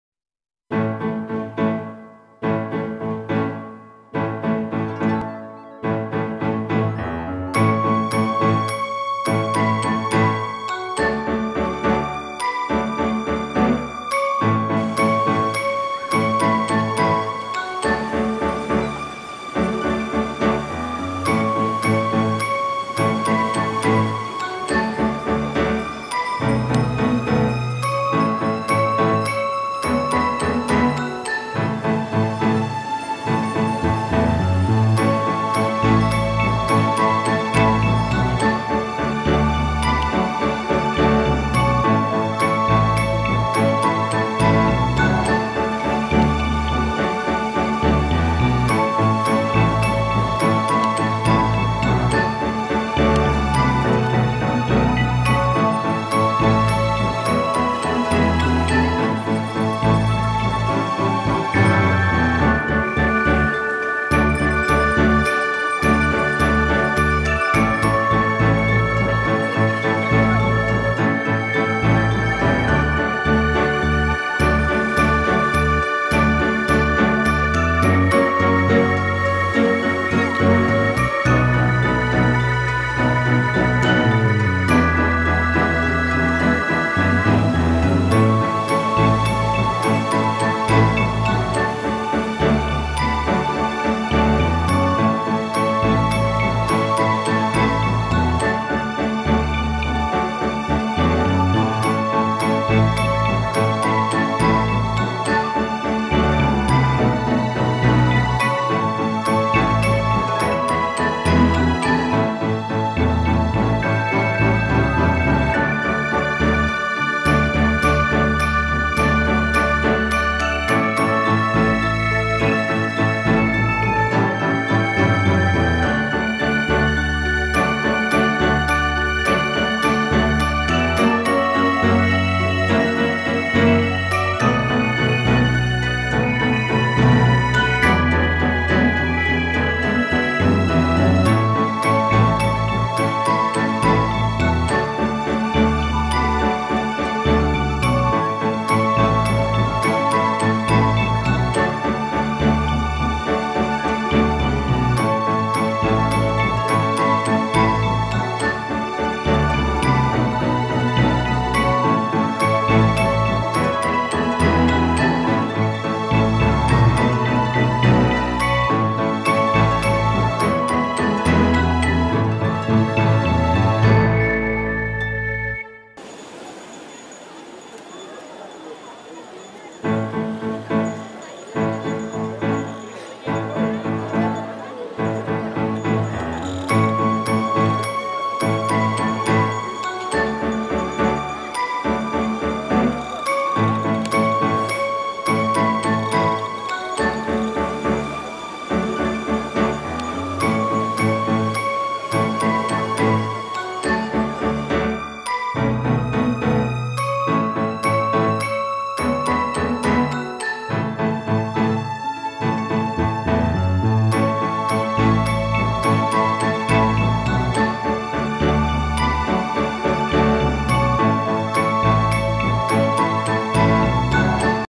Kanyakumari Sunrise and Sunset | Trip to the End point | Vlog
Kanyakumari, (formerly known as Cape Comorin) is a city of the Kanyakumari district in the Indian State of Tamil Nadu. It is the southernmost city of peninsular India. Kanyakumari has been a city since the Sangam period and is a popular tourist destination. Thousands of people came here on daily basis to see the sunrise and sunset happening here.